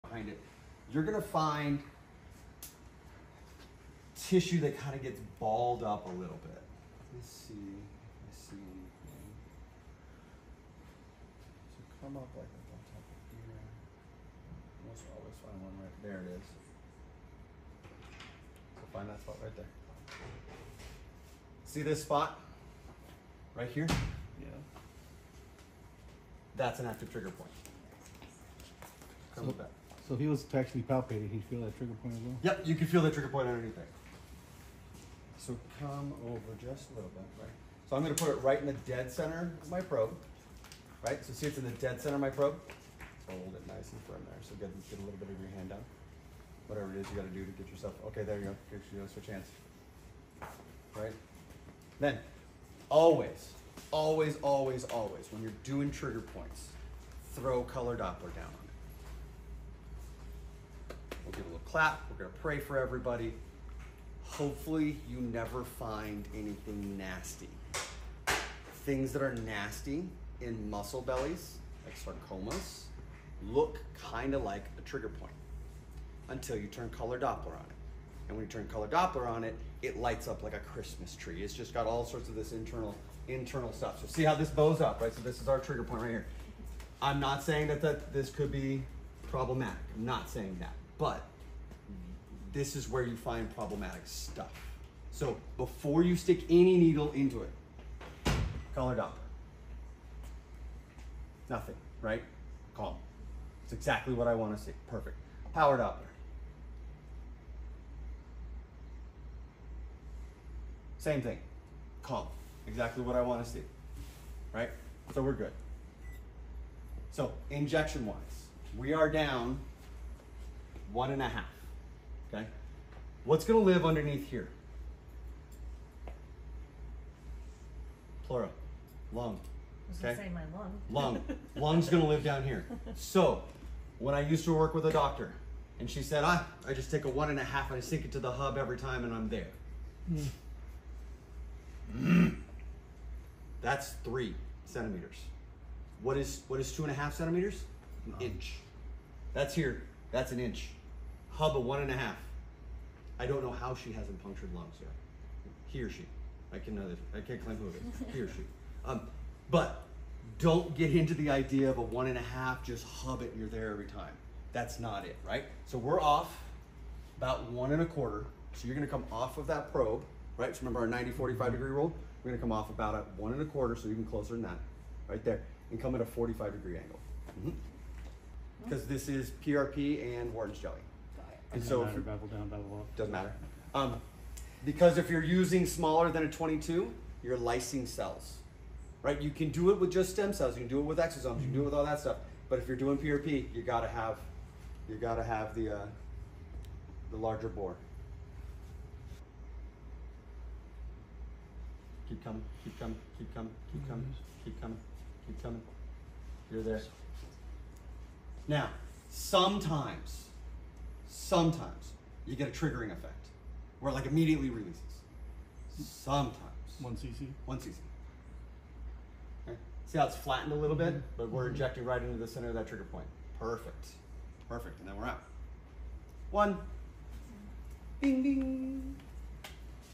Learn Ultrasound Guided Trigger Point Injections with R3
R3 Medical Training teaches how to perform ultrasound guided trigger point injections at the MSK Ultrasound Guided Injection Training Course.

While most providers do trigger point injections blindly, there are a few considerations. One is that there could be a malignancy present, so doppler should be checked. And the other issue is that if a provider goes too deep, it may puncture the lung!

So ultrasound guidance is preferable, and patients will appreciate it too. The R3 Ortho Ultrasound Injection Course is thorough, hands on, and each attendee receives a procedure too! The courses are offered in Scottsdale/Phoenix AZ, Nashville TN and Los Angeles/Beverly Hills CA monthly.